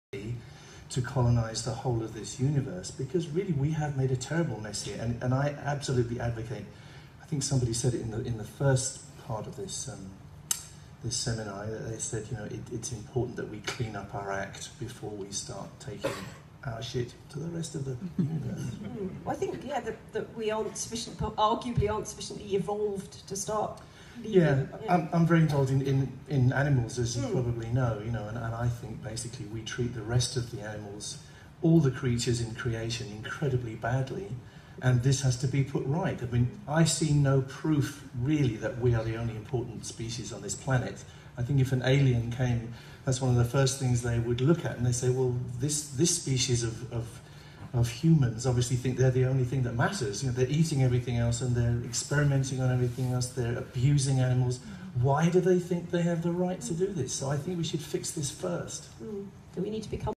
Brian May speaking at Space Rocks 22 April 2018
Brian speaks on the importance looking after our Earth before looking to other planets. TRANSCRIPT
Credit and thanks to Julie Chandler.

An Official Brian May Channel MORE VIDEOS BRIAN'S SOAPBOX OFFICIAL FACEBOOK